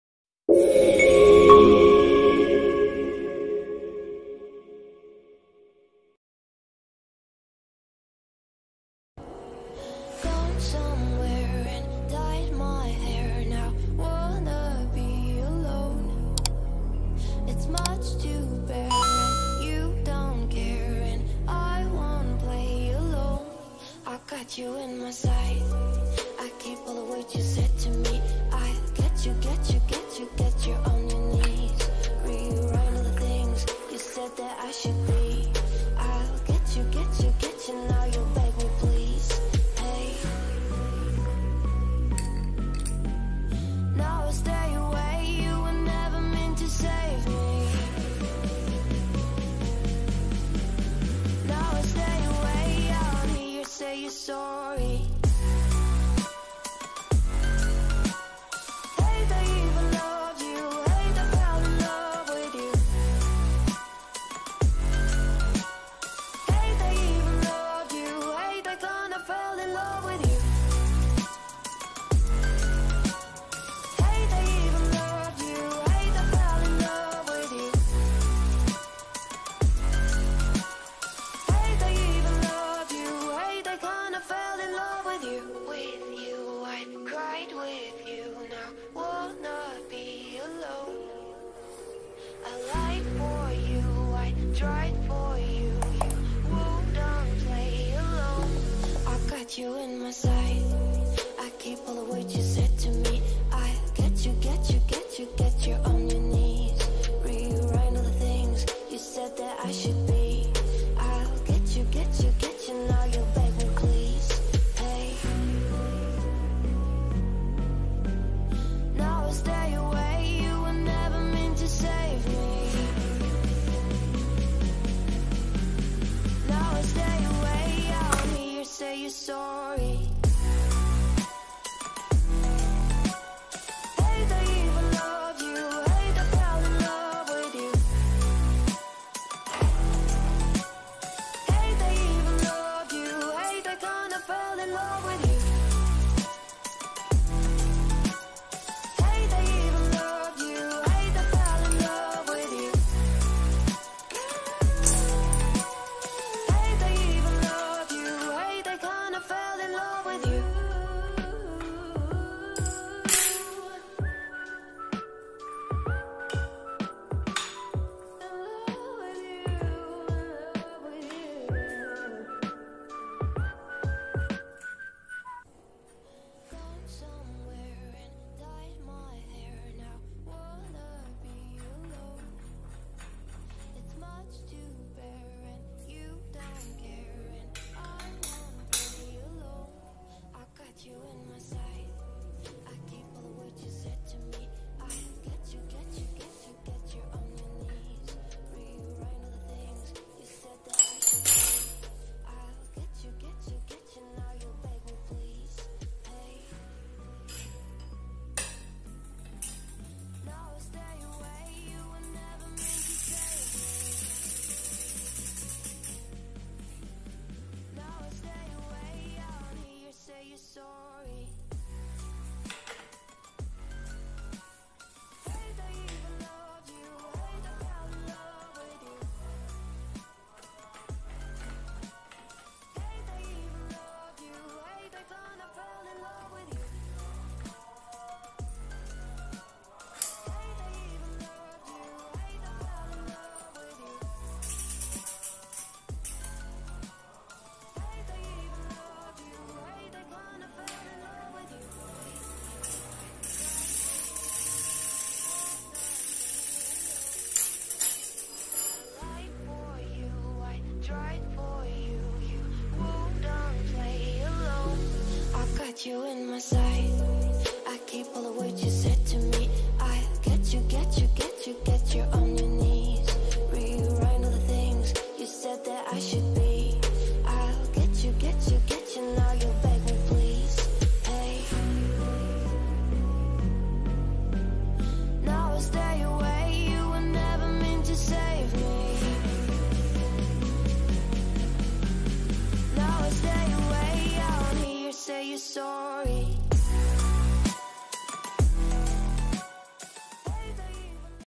PEDAL SEPEDA LIPAT LITEPRO K3 & CARA PASANG PEDAL SEPEDA
Pedal LITEPRO K3 Bearing Ultra Light Alloy
warna : hitam

Berikut video lainnya
Semoga sangat membantu😁👍